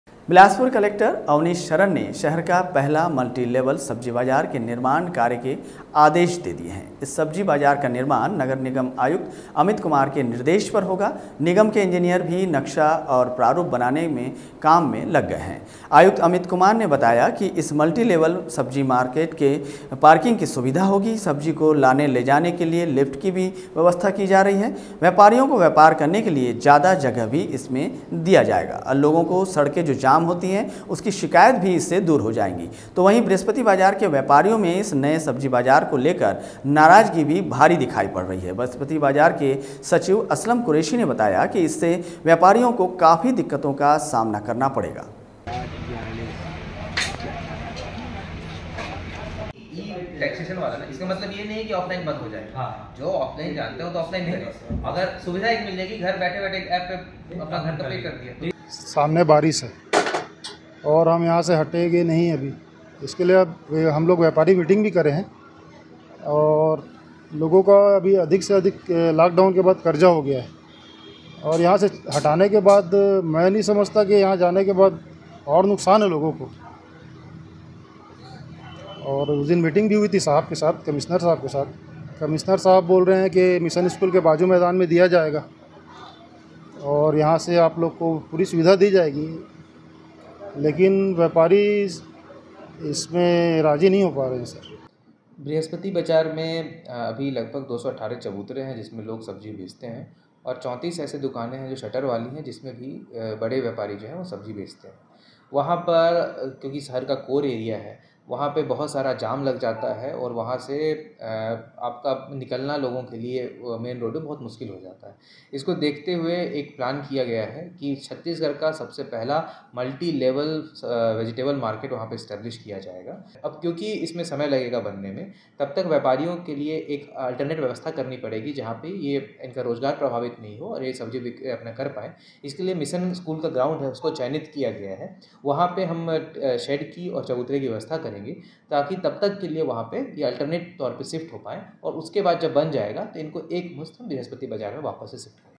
बिलासपुर : कलेक्टर ने दिये मल्टी लेवल सब्जी बाजार बनाने के निर्देश , नक्शा और प्रारूप पर होगा काम
Raftaar Media